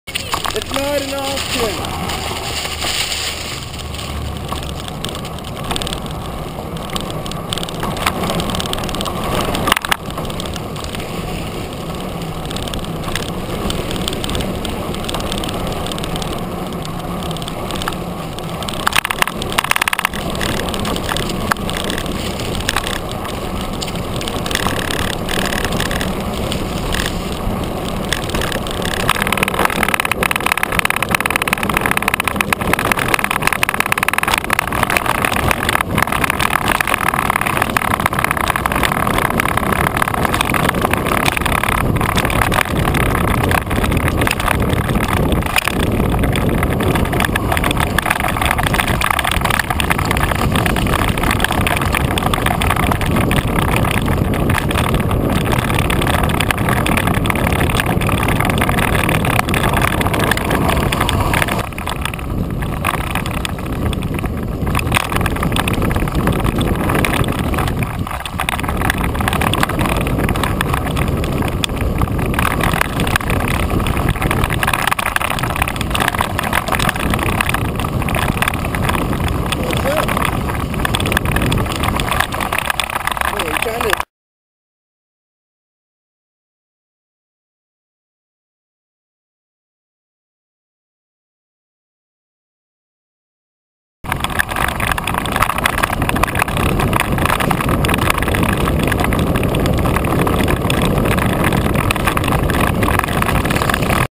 GoPro Fast Mountain Biking w/ Suspension Attachment
I was just testing out the new front shock attachment for my GoPro by riding around a nearby trail, its a really cool camera angle so tell me how you like it! As usual, this video is filmed in 1080p HD 60 frames per second and my bike is a Gary Fisher / Trek Rumblefish 29 inch